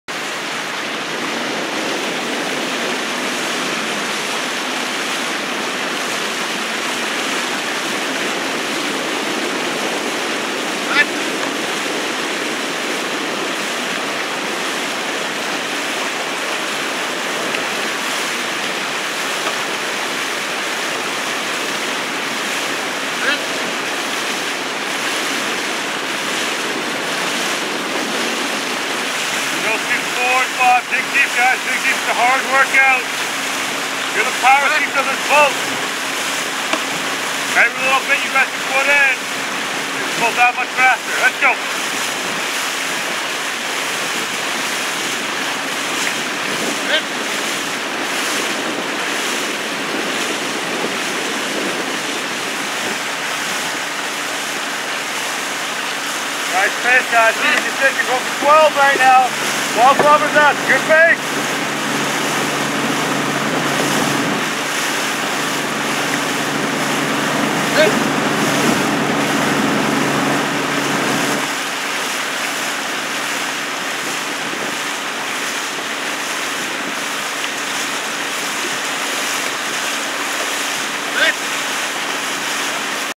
MVI 0251 Ron's Boat
Practice with Dion Maxwell December 13, 2014
Ron's Boat:  Andrew, Edgar, Rob, Brendan, Arthur & Ron C.